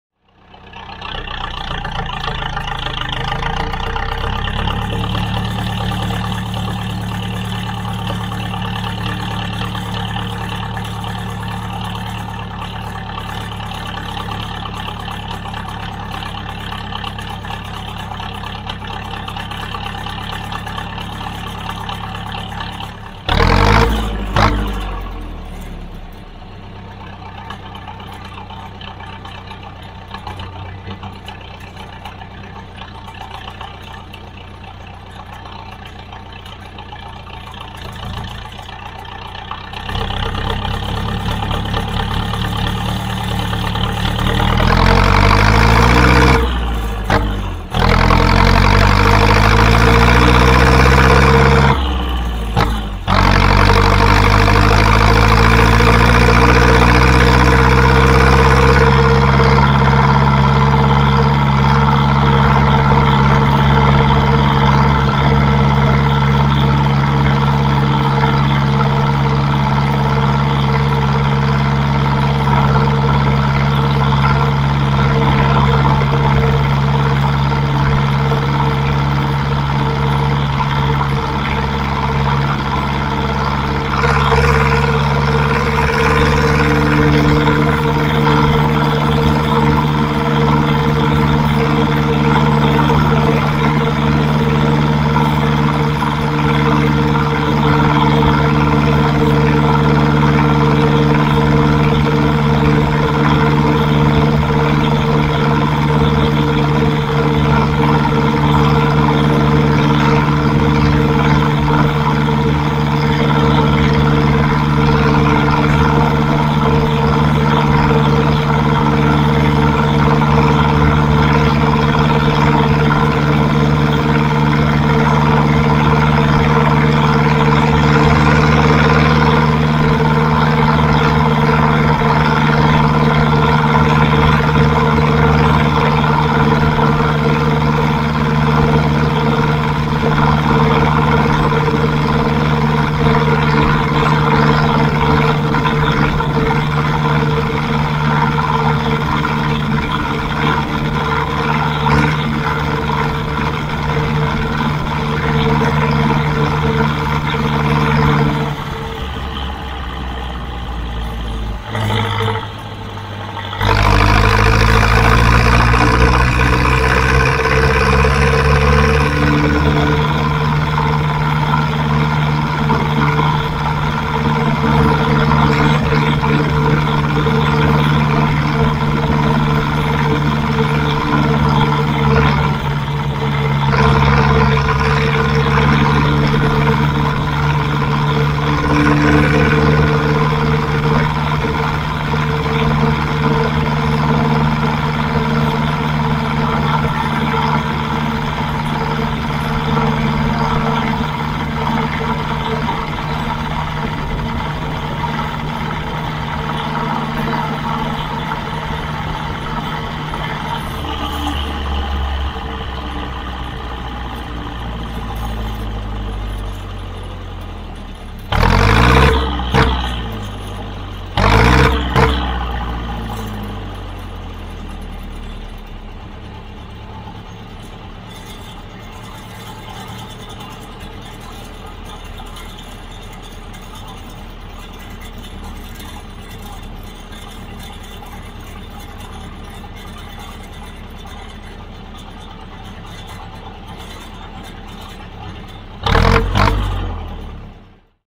DJI OSMO ACTION | Zetor 7211 - Odvoz dřeva z lesa | ORIGINAL ENGINE SOUND
CZ: Ahoj, máme tady další video.. tentokrát je to zvuk traktoru Zetor 7211 s hranatým smaltovaným výfukem při odvozu dřeva z lesa. Posaďte se a vychutnejte si tento nádherný zvuk. :)

EN: Hi, we have another video here .. this time it's the sound of a Zetor 7211 tractor with a square enameled exhaust when removing wood from the forest. Sit back and enjoy this wonderful sound. :)

PL: Cześć, mamy tu kolejny film.. tym razem jest to odgłos traktora Zetor 7211 z kwadratowym emaliowanym wydechem podczas wywożenia drewna z lasu. Usiądź wygodnie i ciesz się tym wspaniałym dźwiękiem. :)